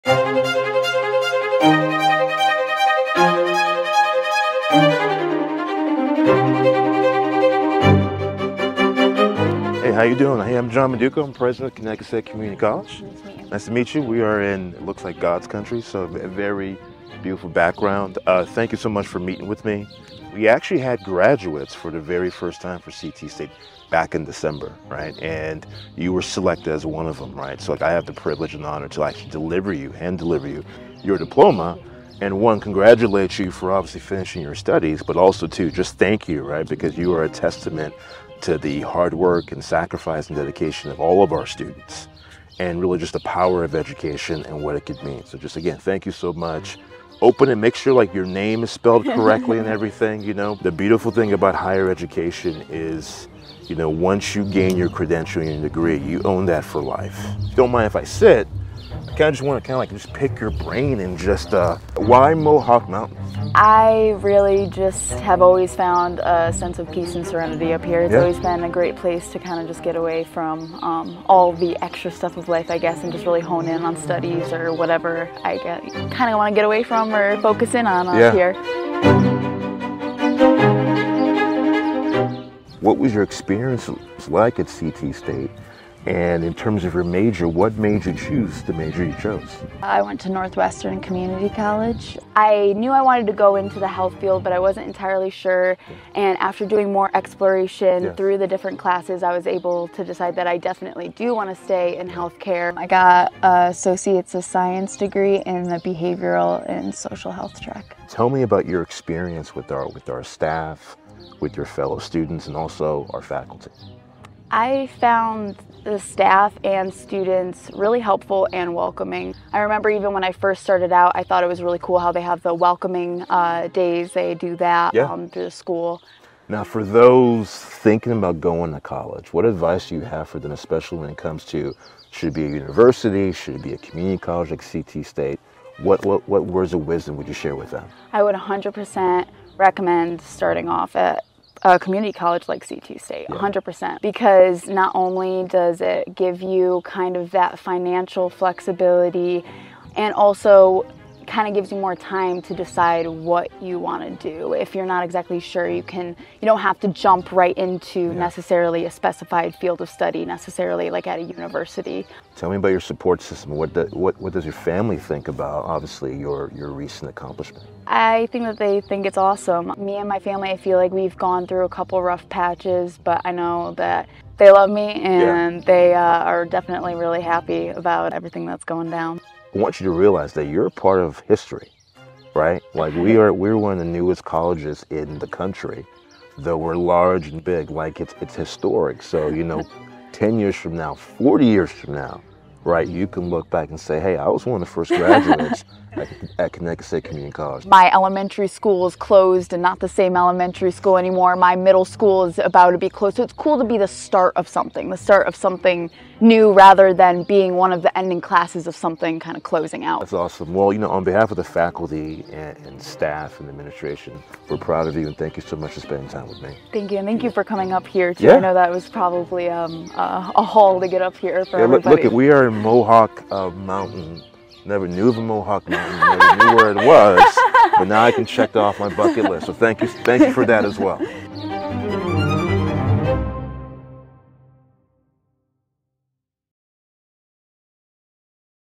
Dr. Maduko Presents Sage Colucci with a Degree in General Studies
President Maduko traveled to Mohawk Mountain to meet with Sage Colucci, who graduated with a degree in General Studies. Sage studied at CT State Northwestern and found solace at the mountaintop when she needed to refocus on her studies. She felt supported throughout her academic career and highly recommends attending a community college. Congratulations, Sage!